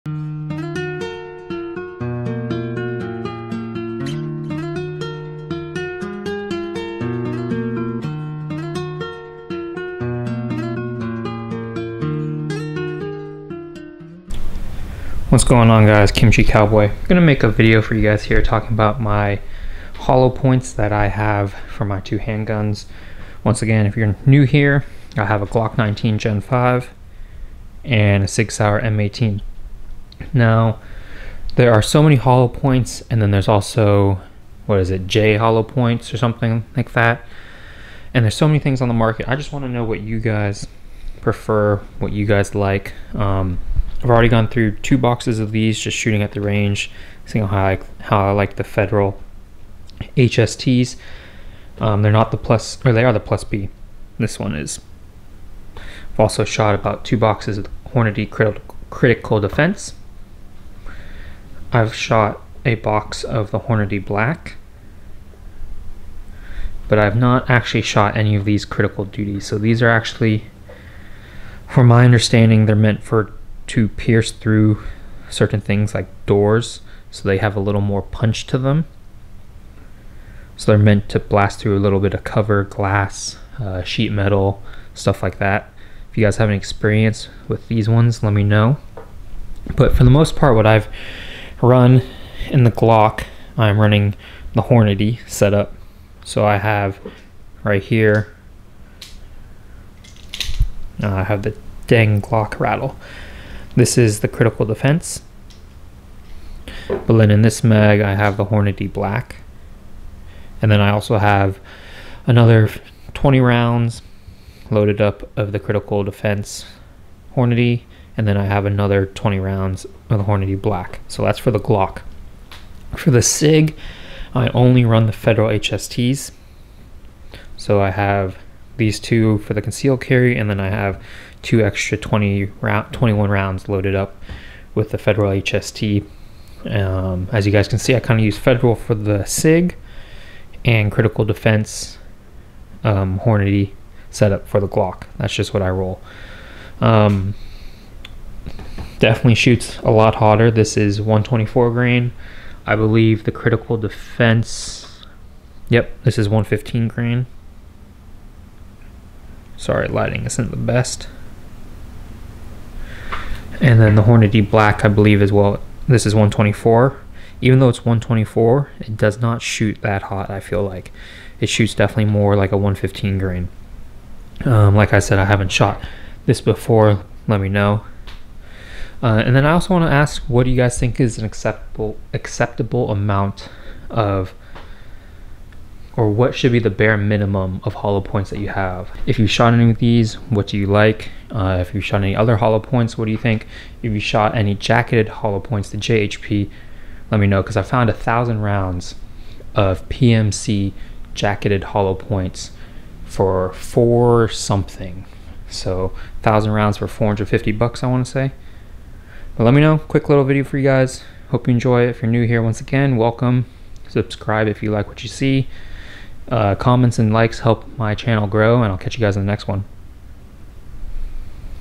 What Hollow Points for Concealed Carry? - 9mm
Today a quick video on the HP I own currently for my 9mm handguns. Curious to see what y'all like or if you run what I run. Let me know in comments down below.